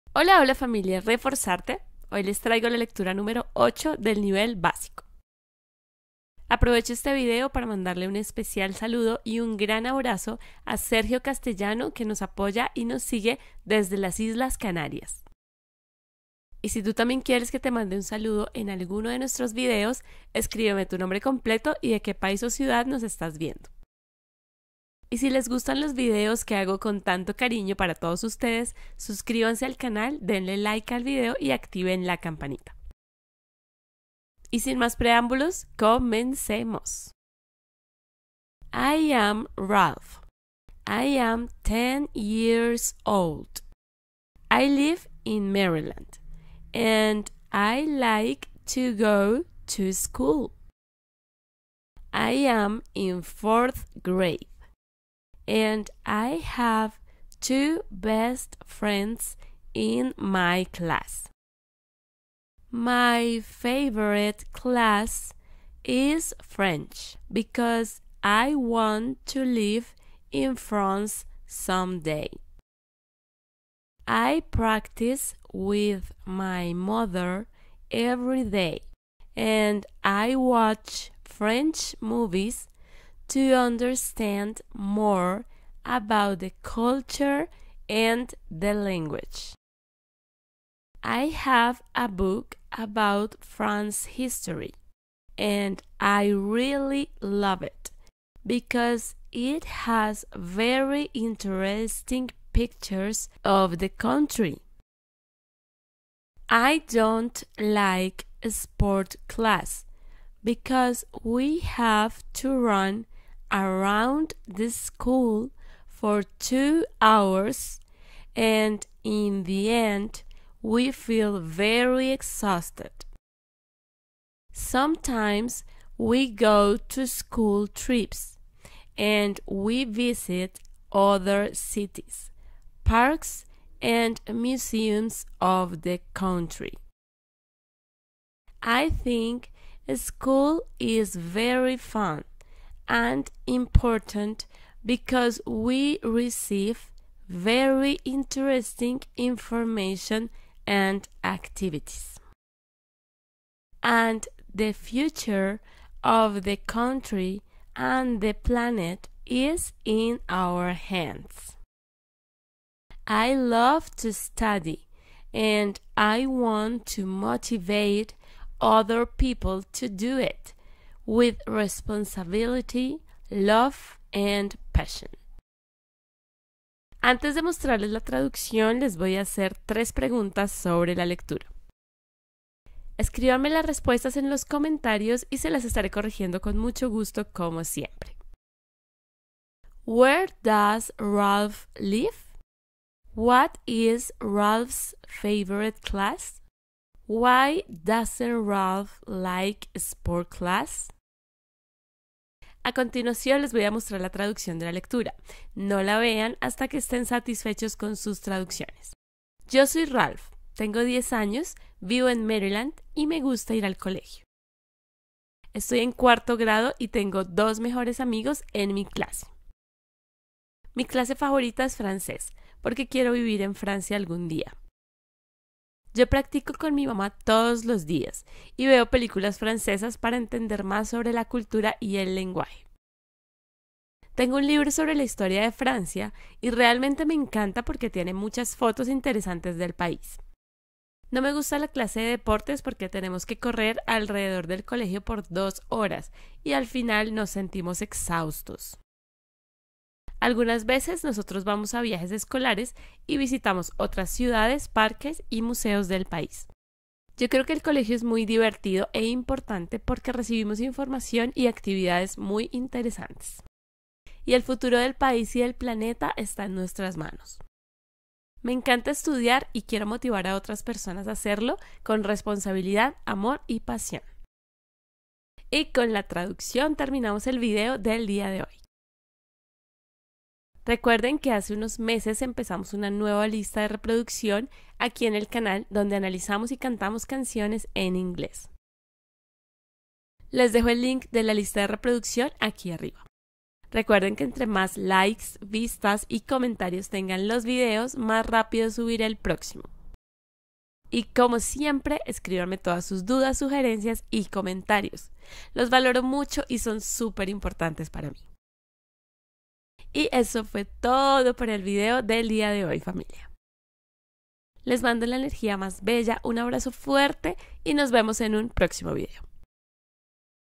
Lectura Nivel Básico #8
Hola hola familia! ... Aquí les traigo la octava lectura del nivel básico para que pongan en practica lo que han aprendido hasta ahora en el curso de Inglés gratuito del canal! :D
Escríbanme las respuestas de las preguntas que les dejé al final del video, se las estaré corrigiendo con muchísimo gusto como siempre!

Y en nuestra página oficial puedes ver todas las materias que dictamos, testimonios de nuestros alumnos, lecturas y muchas más!